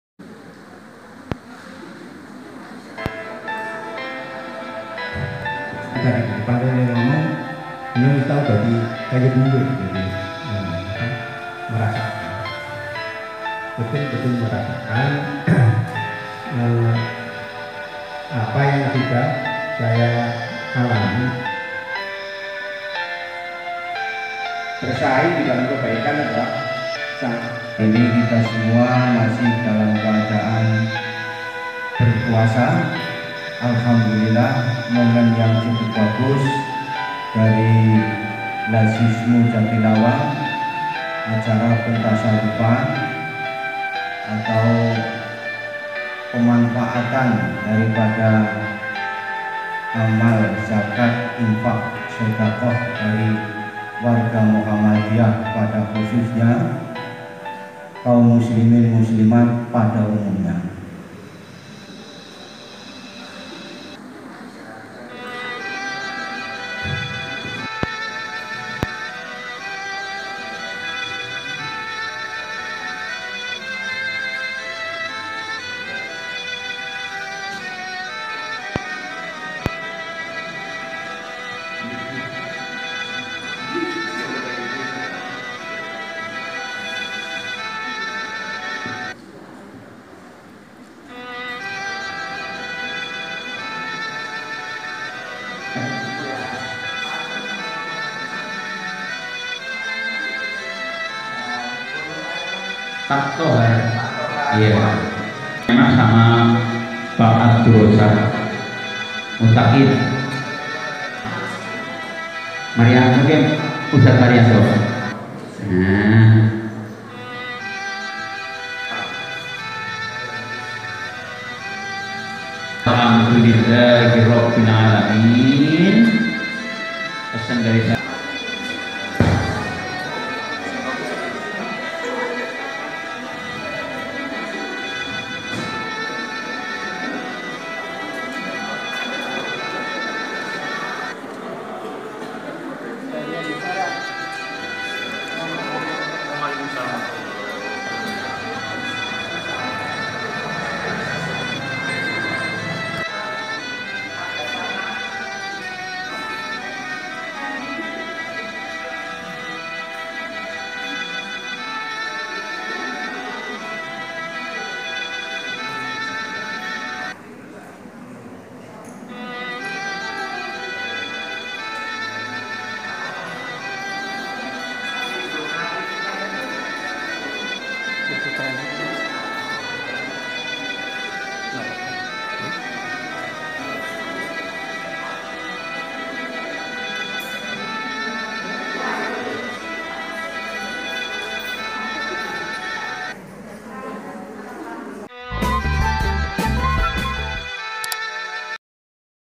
INDAHNYA BERBAGI, LAZISMU JATILAWANG#Nada Syahdu
Indahnya#Berbagi#Indahnya Berbagi# Lazismu Jatilawang#Program Bantuan untuk para guru Bakti baik Paud,TK,MI,SMP dan juga pada Ustad Ustadzah